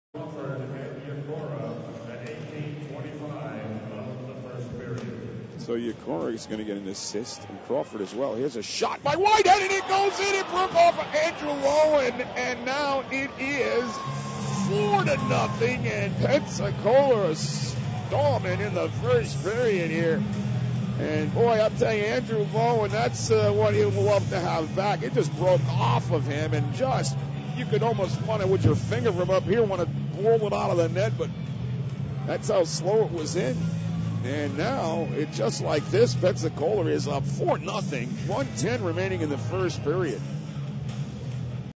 Flyers g4 Whitehead.flv
IceFlyers goal scored by Matt Whitehead at the 18:50 mark of the 1st period. IceFlyers lead the Snakes 4-0 and the rout is on. game date 1/1/11